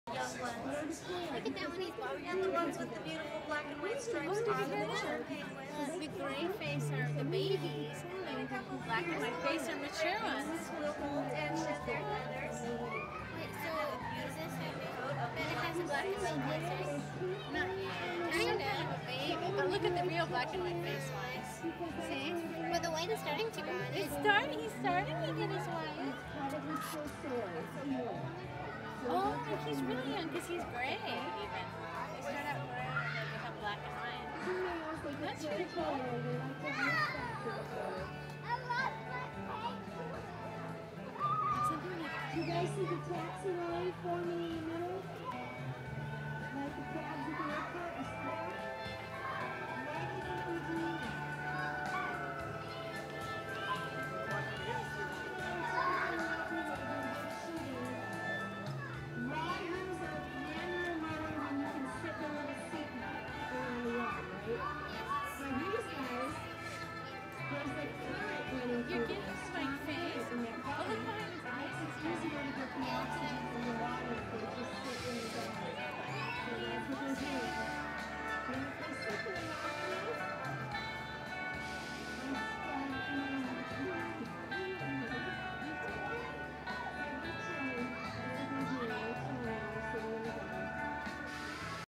Six Flags Marineworld (Vallejo, CA USA; MVI 1022)
Our annual trip to Six Flags MarineWorld Africa USA in Vallejo, California.